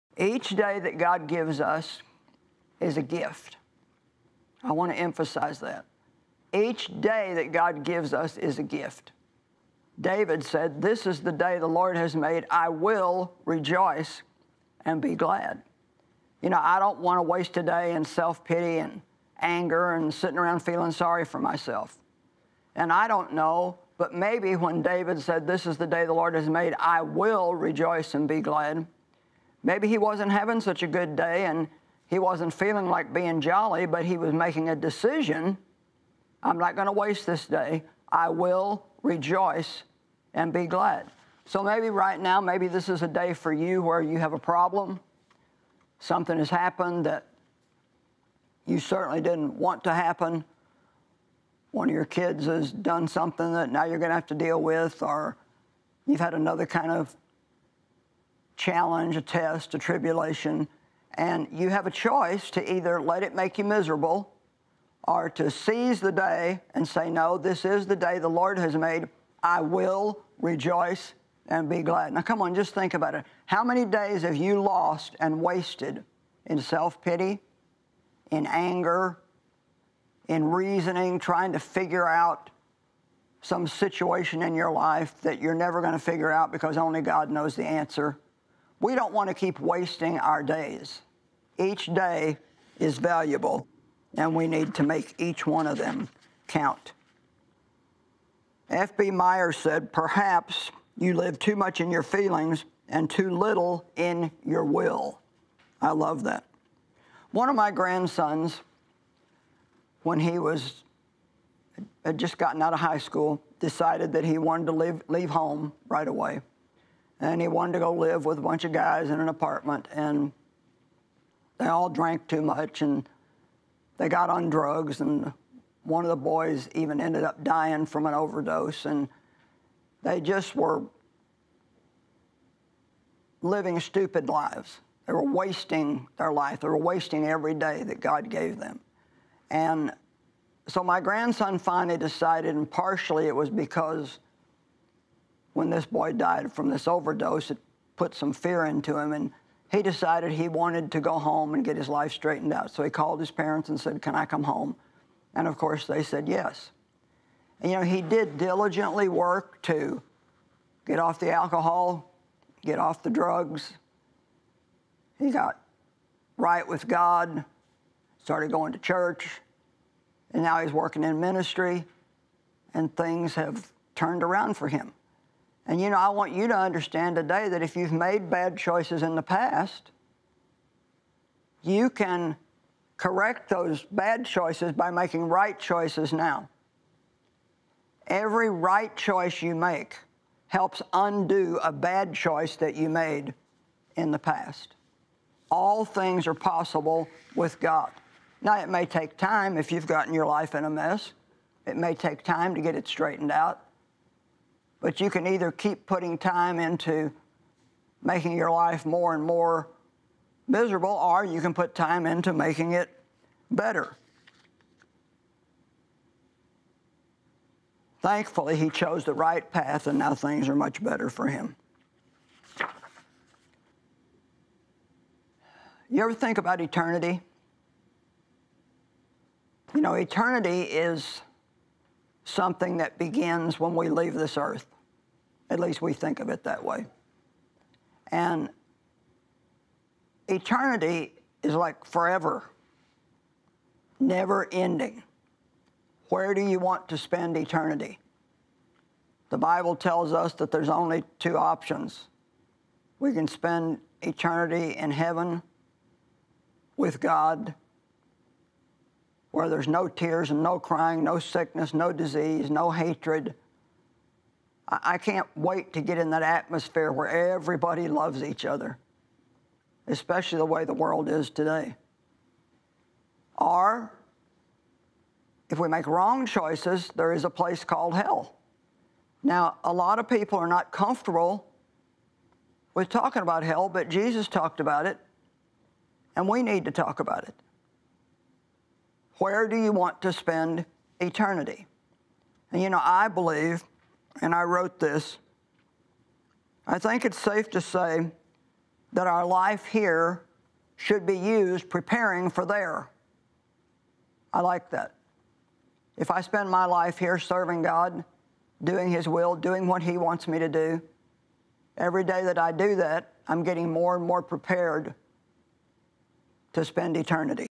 Today Is A Day To Rejoice | Joyce Meyer
Are you having a bad? Facing something difficult? As Joyce reminds us, we have choice to either stay miserable or we can be like David and say, "This is the day the Lord has made, I will rejoice and be glad."